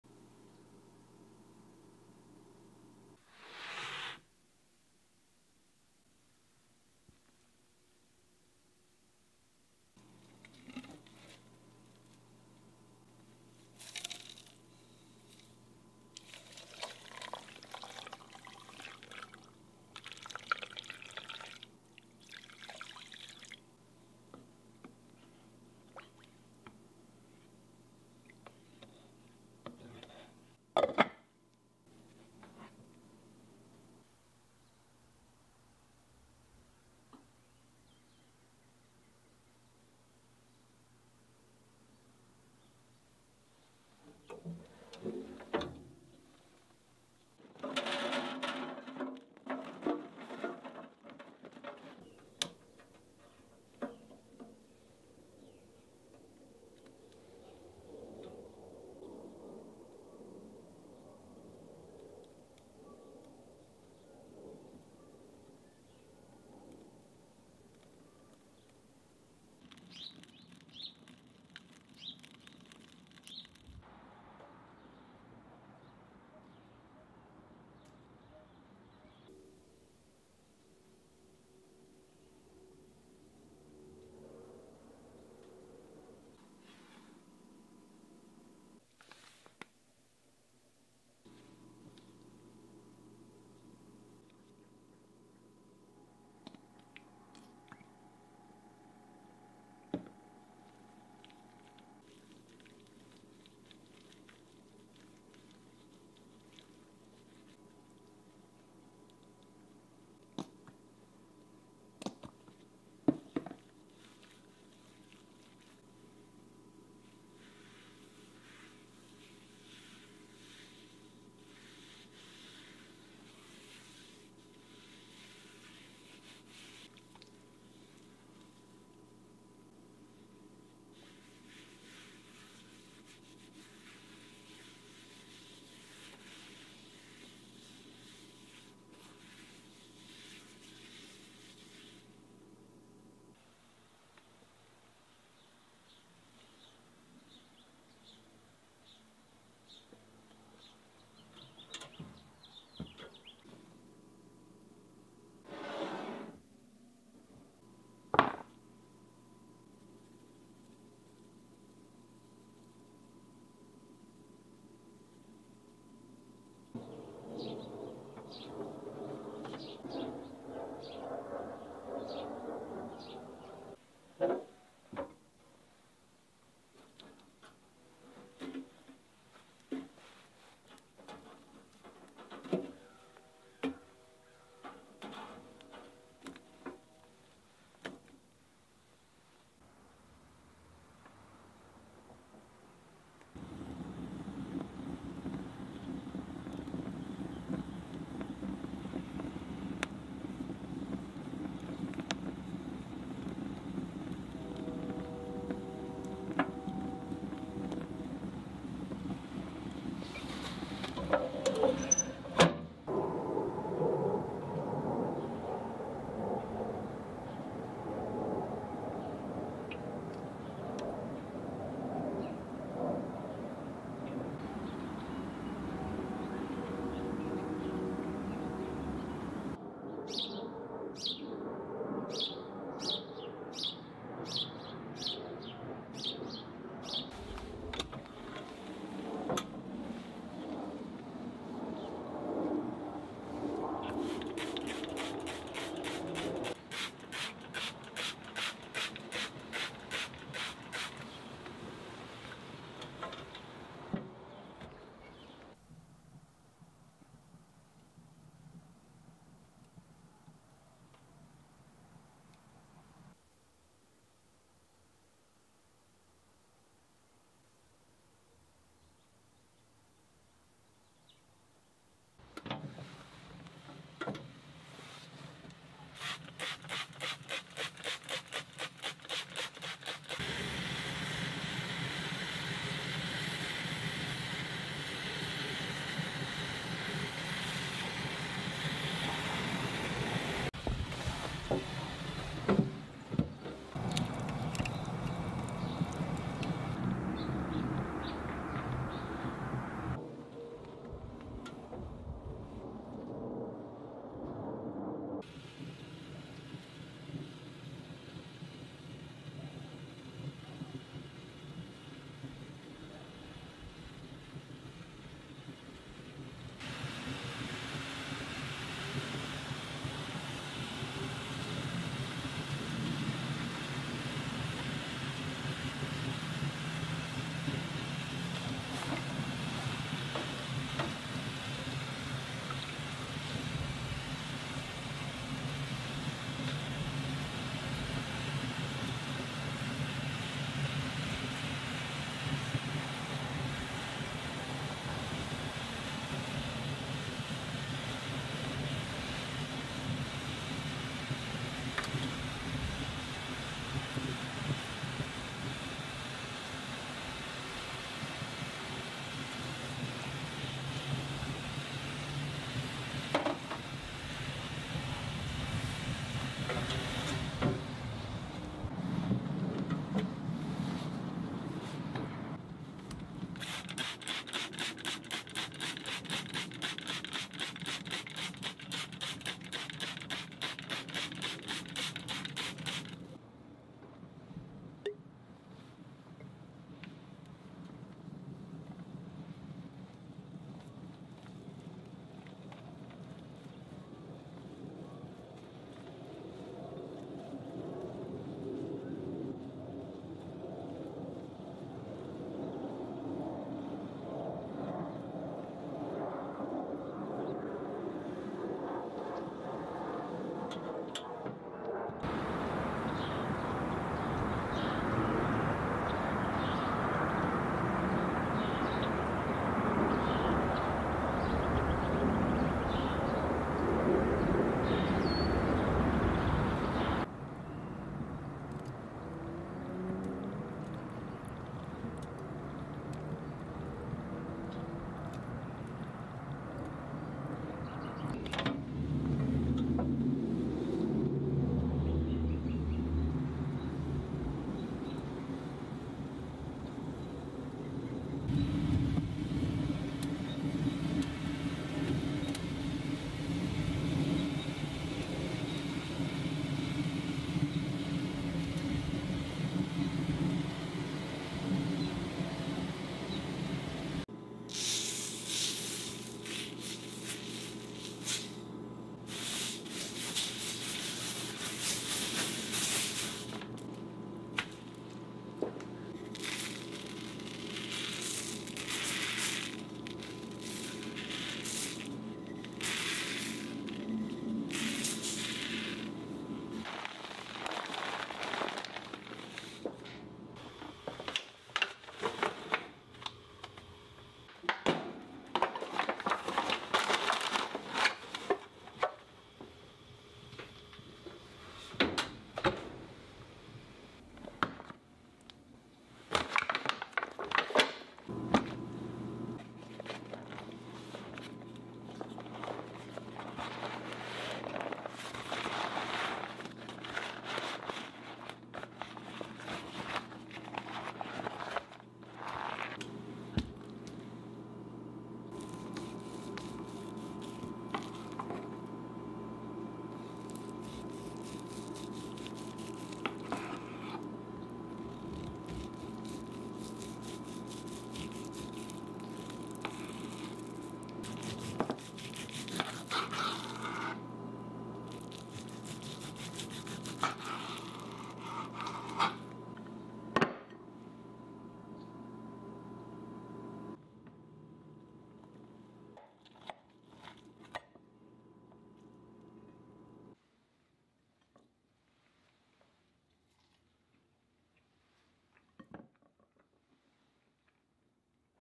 Smoked Ribs and Pork Shoulder
Smoked meat is the result of a method of preparing red meat, white meat, and seafood. Smoking adds flavor, improves the appearance of meat through the Maillard reaction, and when combined with curing it preserves the meat.